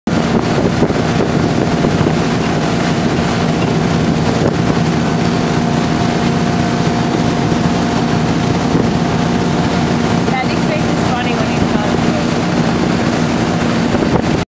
Slalom Skiing on Indian Lake
Tom takes a break from driving everyone around to do some skiing!